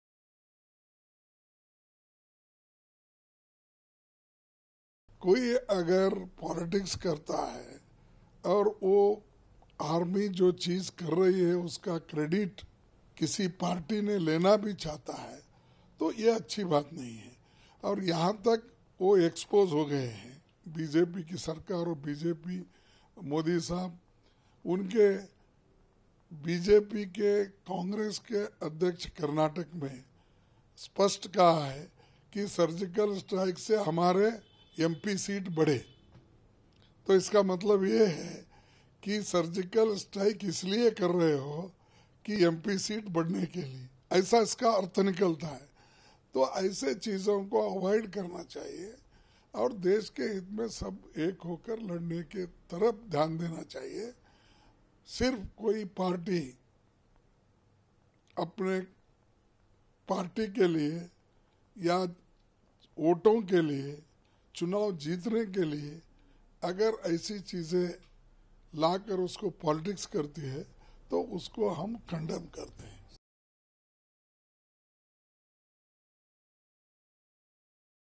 It is not good if someone is doing politics on the work of Army: Mallikarjun Kharge
Bengaluru, Mar 04 (ANI): While talking to ANI Congress leader Mallikarjun Kharge on Monday said, “ 'I don’t understand why some people still want to question forces. Nobody is questioning, Modi himself is questioning, he wants to take credit and we support our Army. We shouldn't learn patriotic lessons from Modi.”  He also said that BJP is exposed now they are doing surgical strike for their political benefits. We should stand united in this situation and we should focus on national interest now, said Kharge.